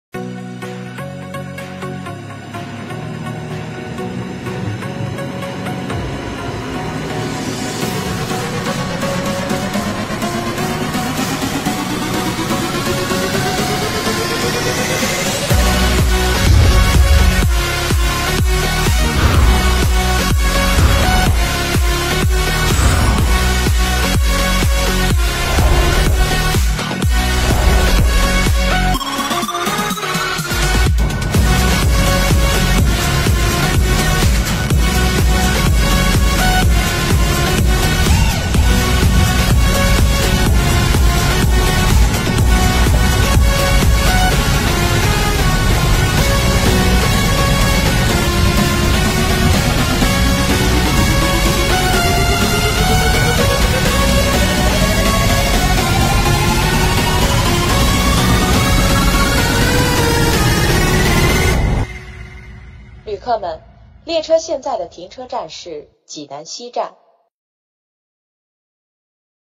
897KM long, unique perspective of Chinese high-speed rail drivers 897公里长中国高铁司机独特视角
This video taken from the driver's perspective along the 897 kilometers long of the high-speed railway loop in Shandong province gives a unique viewpoint while the high speed train passes tunnels, viaducts and bridges.  分享视频来源CRRC